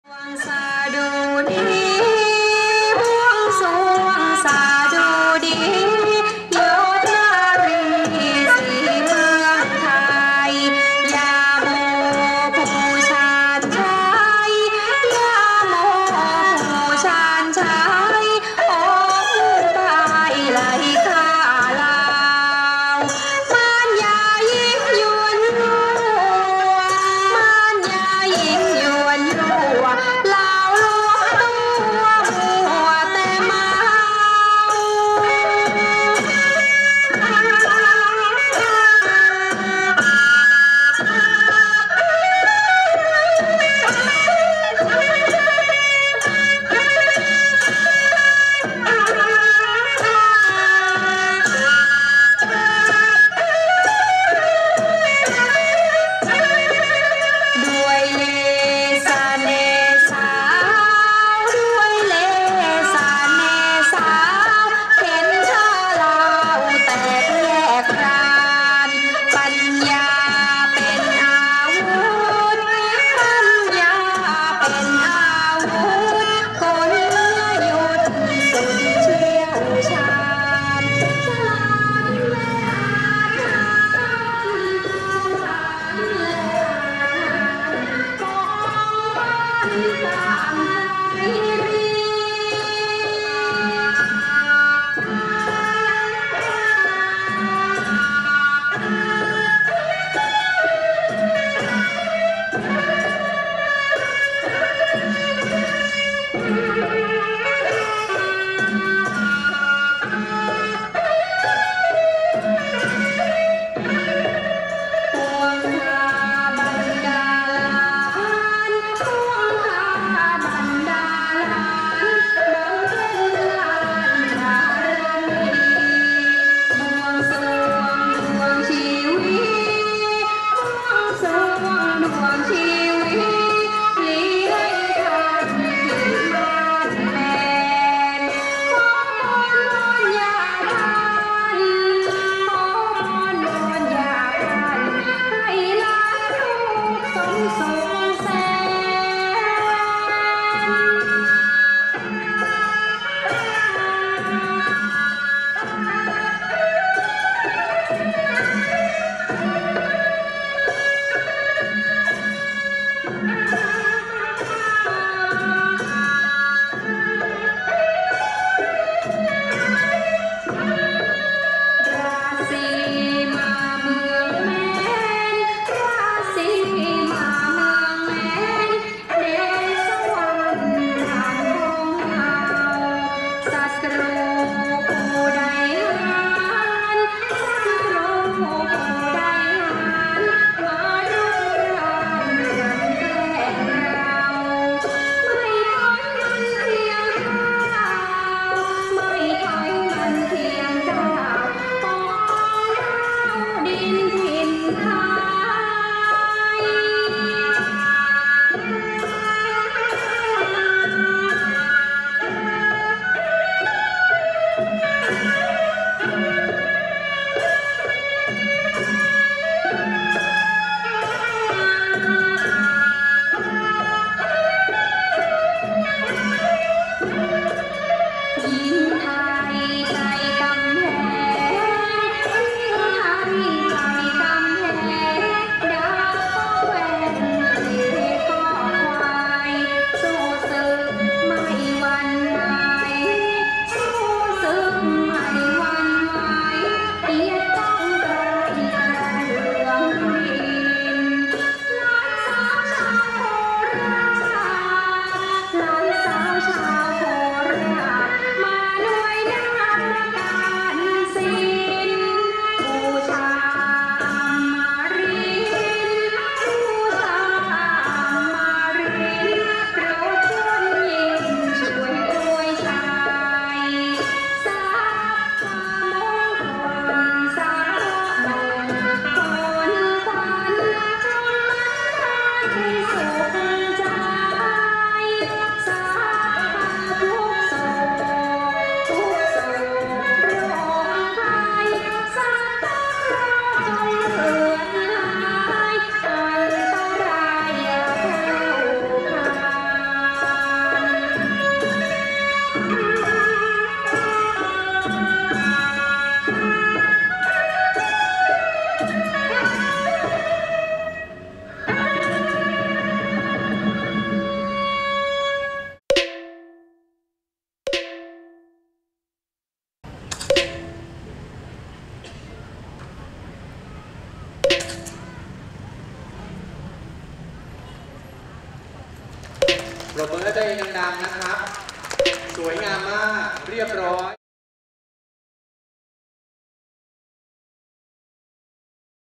ซ้อมรำบวงสรวงท้าวสุรนารี 22มีนาคม จังหวัดนครราชสีมา2560 FullHD1080P
ซ้อมรำบวงสรวงท้าวสุรนารี วันที่22มีนาคม จังหวัดนครราชสีมา2560 FullHD1080P  ตำบลในเมือง จังหวัดนครราชสีมา งานเริ่มจริงวันที่23มีนาคม2560  ถึง3เมษายน2560 เป็นประเพณีที่ยิ่งใหญ่อลังการ ของชาวเมืองนครราชสีมา   ภาพในมีการสั่นไหวต้องขออภัยท่านผู้ชมด้วยนครับ คนเยอะมาก "ขอขอบคุณครับสมาชิกที่ติดตามชม.